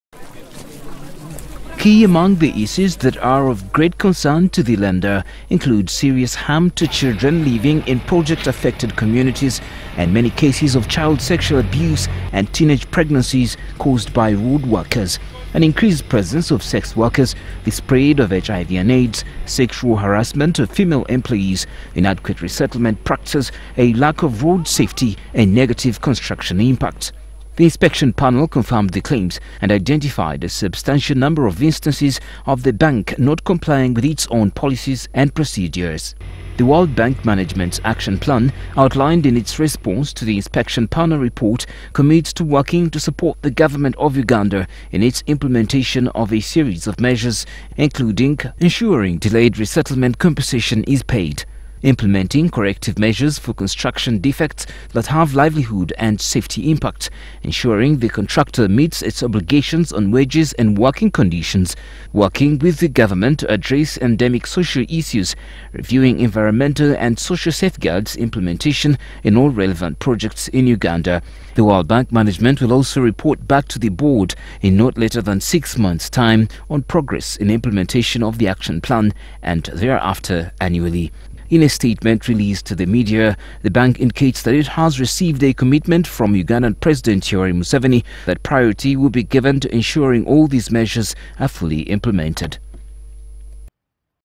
World Bank gives Uganda six months to work towards having funding suspension lifted
Uganda has six month to act on five world bank recommendations to enable a lifting of suspension of funding on two civil works projects in the Albertine region and in the North East of the country. Delay in compensation to affected communities along road projects was one of several reasons the world bank suspended funding. Its however not clear when the entire world bank suspension on all future funding will be lifted over issues of low project fund absorption